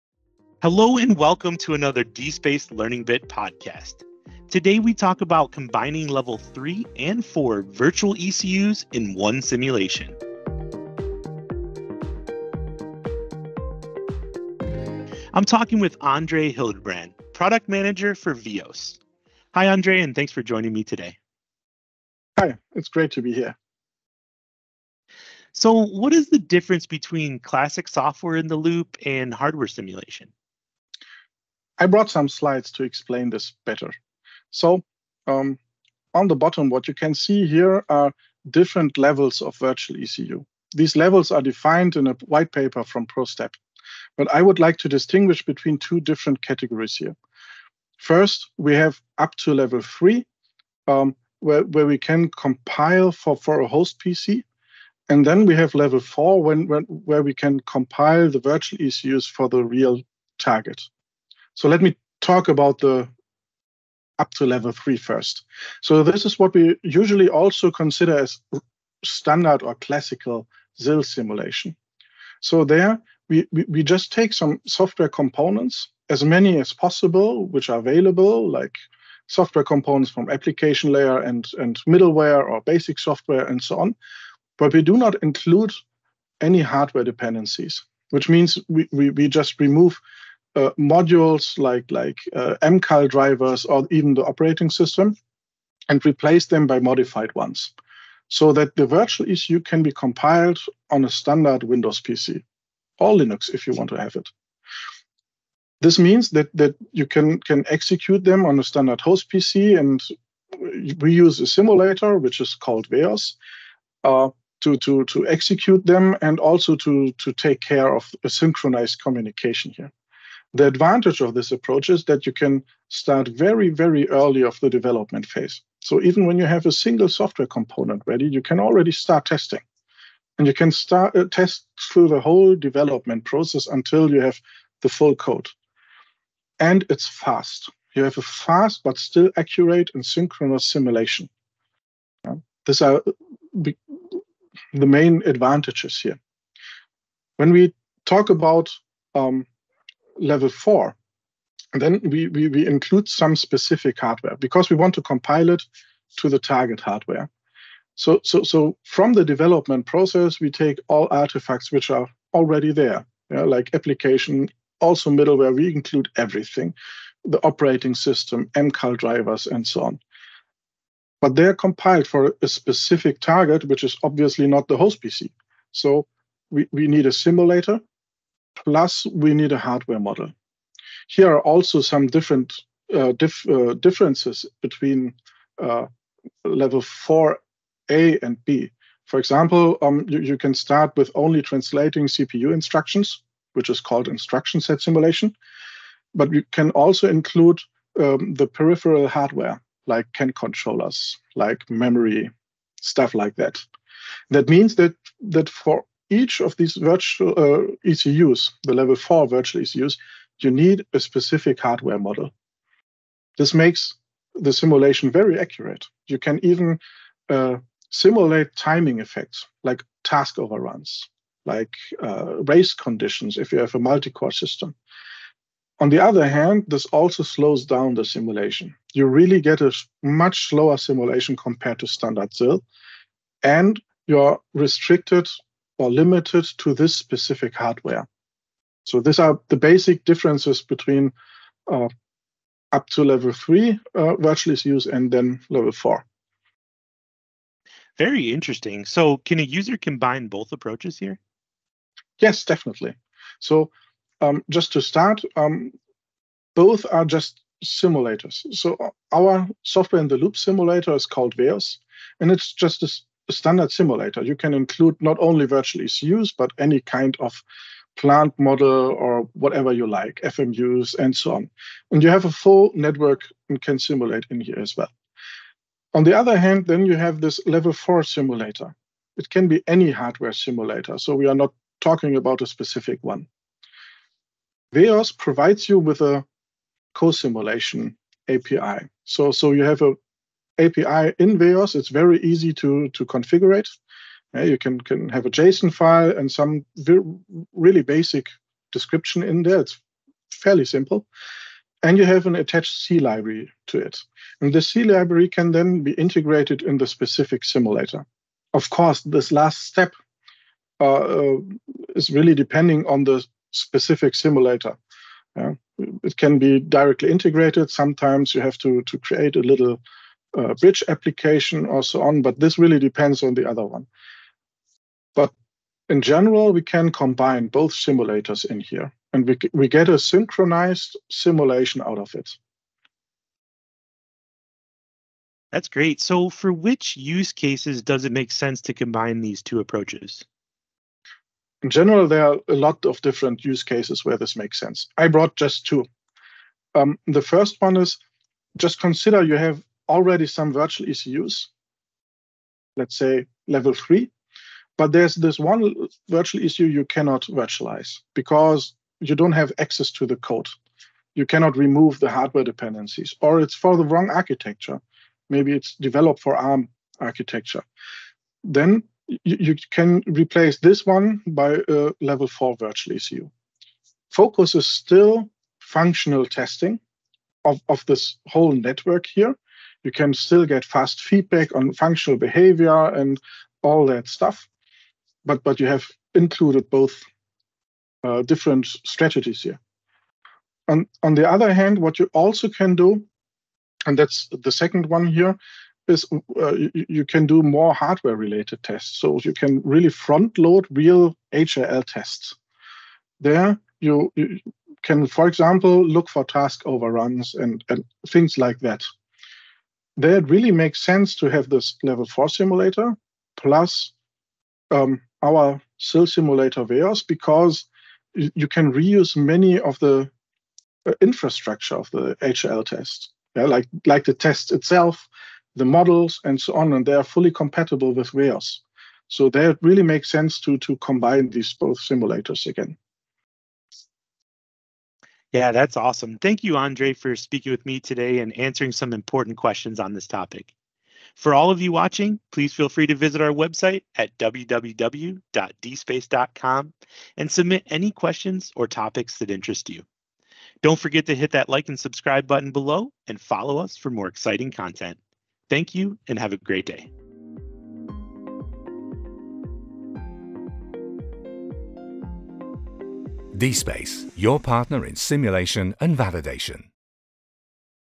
Combining Level 3 and 4 Virtual ECUs in One Simulation
The combination makes the difference.
Watch our new Learning Bits episode and learn how to combine Level 3 and Level 4 virtual ECUs in one simulation.

In the virtual world, you don’t have to wait for the hardware ECU to become available to start testing the software. Using virtual electronic control units (V-ECUs), you can start testing your ECU software during the very early development stage in the cloud or on your PC.

Andre Hildebrandt, our Product Manager for VEOS, gives an overview of the different levels of virtual ECUs and explains how to combine software-in-the-loop simulation with hardware simulation.